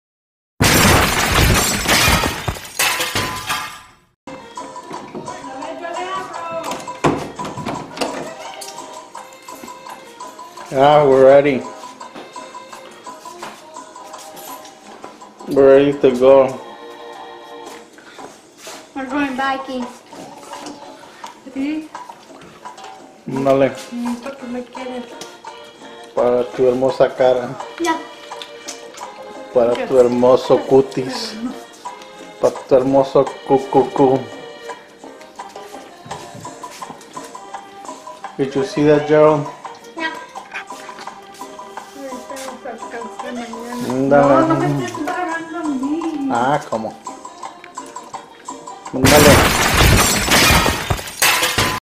Life in Yuma, AZ (Part 1)
I'm having fun with my camcorder.

On this video Jared and I are going on a bike rid, and we are trying to convince Daisy to go with us.  But she doesn't want to because she wants to stay home and watch her novelas (soap operas).